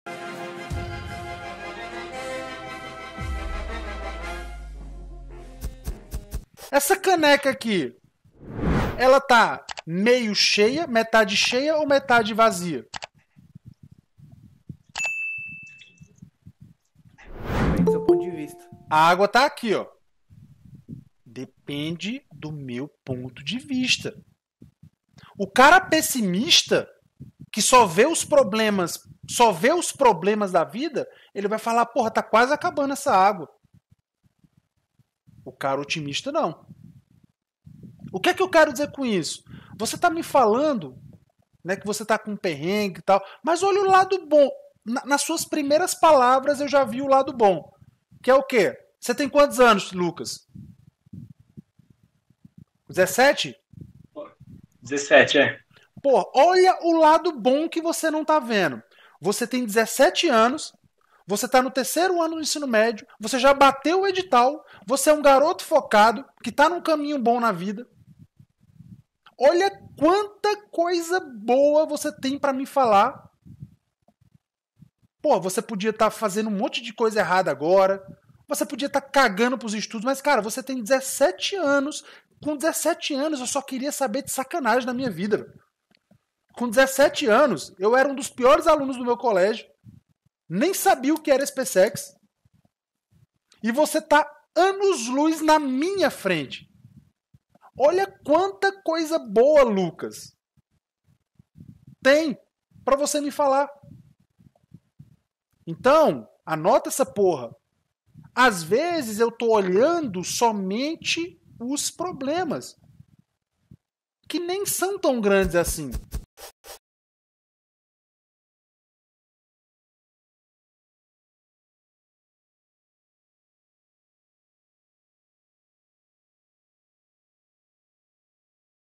COMO LIDAR COM OS MOMENTOS DIFÍCEIS DA PREPARAÇÃO | MENTORIA ELITE MIL
Um bizu do Tenente Thiago Henrique para você que muitas vezes só consegue ver o lado ruim da sua situação!

Esse é um trecho do encontro semanal com alguns dos alunos do Elite Mil, um momento de conversa com o Tenente ou com o Capitão Cacavo, para falar sobre as maiores dificuldades do dia a dia de um concurseiro.

Deixe aqui nos comentários o "seu lado bom", vamos motivar uns aos outros!

💬 Fique por dentro de tudo sobre os concursos militar, entre para o Grupo Elite Mil para receber conteúdos e avisos importantes